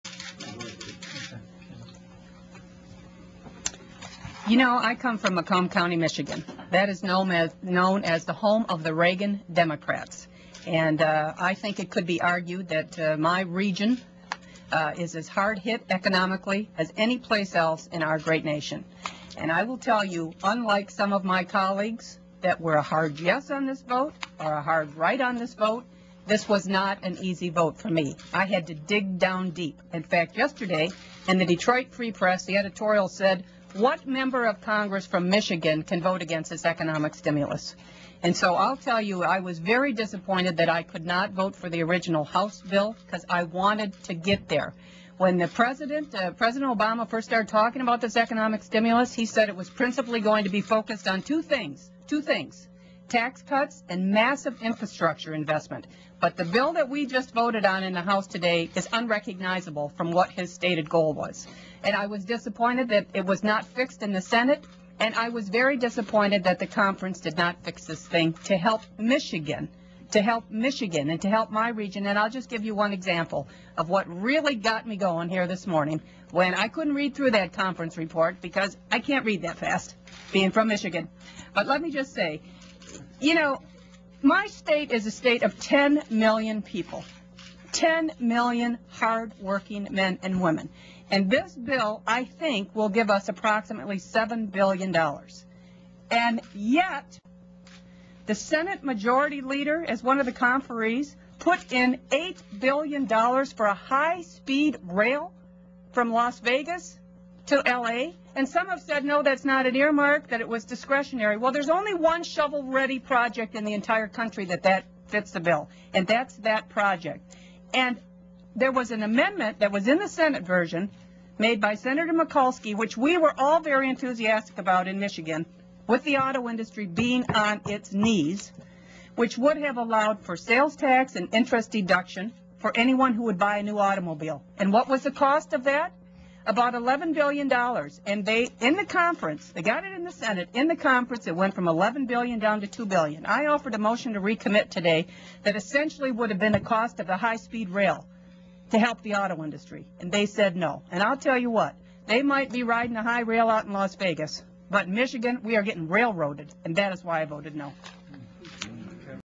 Rep. Miller speaks at GOP Stimulus Press Conference
REP. CANDICE MILLER:

You know, I come from Macomb County, Michigan. That is known as the home of the Reagan Democrats. And I think it could be argued that my region is as hard-hit economically as anyplace else in our great nation. And I will tell you, unlike some of my colleagues that were a hard yes on this vote or a hard right on this vote, this was not an easy vote for me. I had to dig down deep. In fact, yesterday, in the Detroit Free Press the editorial said, "What member of Congress from Michigan can vote against this economic stimulus?"

And so I'll tell you, I was very disappointed that I could not vote for the original House bill, because I wanted to get there. When the President -- President Obama first started talking about this economic stimulus, he said it was principally going to be focused on two things. Two things, tax cuts and massive infrastructure investment. But the bill that we just voted on in the House today is unrecognizable from what his stated goal was. And I was disappointed that it was not fixed in the Senate, and I was very disappointed that the conference did not fix this thing to help Michigan -- to help Michigan and to help my region.

And I'll just give you one example of what really got me going here this morning, when I couldn't read through that conference report -- because I can't read that fast, being from Michigan. But let me just say, you know, my state is a state of 10 million people. Ten million hard-working men and women. And this bill, I think, will give us approximately $7 billion.

And yet the Senate Majority Leader, as one of the conferees, put in $8 billion for a high-speed rail from Las Vegas to L.A. And some have said no, that's not an earmark, that it was discretionary.

Well, there's only one shovel-ready project in the entire country that that fits the bill, and that's that project.

And there was an amendment that was in the Senate version, made by Senator Mikulski -- which we were all very enthusiastic about in Michigan, with the auto industry being on its knees -- which would have allowed for sales tax and interest deduction for anyone who would buy a new automobile. And what was the cost of that? About $11 billion. And they, in the conference -- they got it in the Senate -- in the conference, it went from $11 billion down to $2 billion.

I offered a motion to recommit today that essentially would have been the cost of the high-speed rail, to help the auto industry, and they said no. And I'll tell you what, they might be riding a high rail out in Las Vegas, but Michigan, we are getting railroaded. And that is why I voted no.